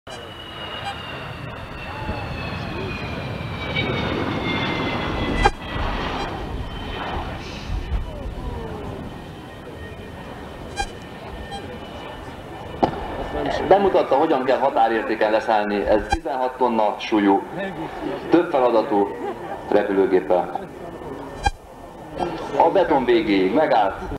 Su-22 drogue-chute landing gone wrong...
Su-22 Hungarian air show, Taszar Airbase 1992